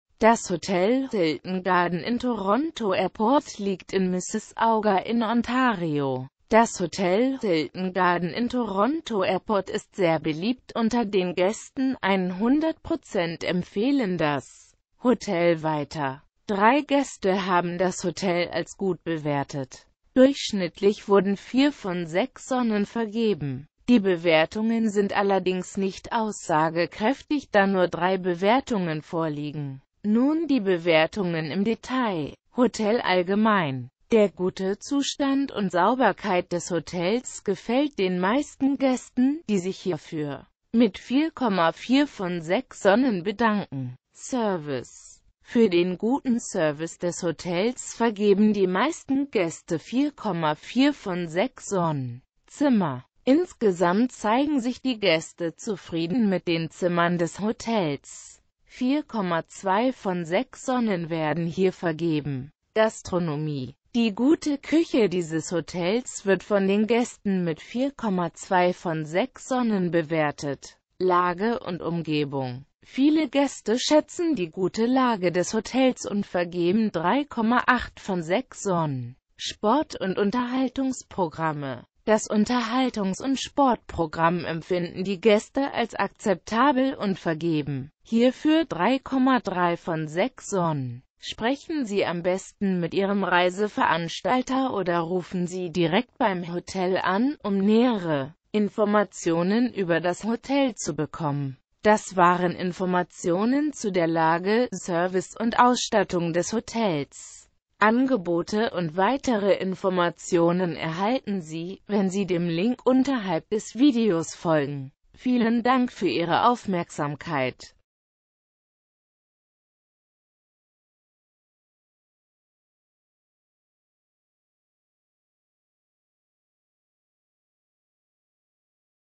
Hotel Hilton Garden Inn Toronto Airport in Mississauga (Ontario - Kanada) Bewertung und Erfahrungen
Das Hotel Hilton Garden Inn Toronto Airport liegt in der Stadt Mississauga welche zu Kanada (Nordamerika) gehoert.

 Das Hotel Hilton Garden Inn Toronto Airport ist sehr beliebt unter den Gaesten, 100% empfehlen das Hotel weiter. Zudem erhaelt das Hotel 4 von 6 Sonnen. Die Hotelbewertungen sind allerdings nicht aussagekraeftig, da nur 3 Bewertungen vorliegen. Sprechen Sie am besten mit Ihrem Reiseveranstalter oder rufen Sie direkt beim Hotel an, um mehr Informationen ueber das Hotel zu bekommen.
Hotel allgemein:
Service:
 Fuer den guten Service des Hotels vergeben die meisten Gaeste 4,4 von 6 Sonnen.
Zimmer:
 Insgesamt zeigen sich die Gaeste zufrieden mit den Zimmern des Hotels. 4,2 von 6 Sonnen werden hier vergeben.
Gastronomie:
 Die gute Kueche dieses Hotels wird von den Gaesten mit 4,2 von 6 Sonnen Bewertet.

Informationen zur Verfuegbarkeit von Transfer zum Hotel, WLAN in den Zimmern des Hotel Hilton Garden Inn Toronto Airport, sowie Details zum Animations-Progamm, Entfernung zum Strand oder zu Diskotheken erhalten Sie bei Ihren Reiseveranstalter oder auf der Webseite des Hotels. Neben den hier verwendeten Bewertungen, lohnt sich ein Blick auf die ggf. vorhandene Hotel Hilton Garden Inn Toronto Airport Facebook-Seite.

Quellenhinweis:
Die verwendeten Informationen stammen von HolidayCheck.de. Sie wurden am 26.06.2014 abgerufen. In diesem Video wurden Bilder aus Mississauga oder allgemeine Bilder aus Kanada verwendet. Die gezeigten Orte und Landschaften dienen als Beispiel und muessen nicht unbedingt in der Naehe des Hotels liegen.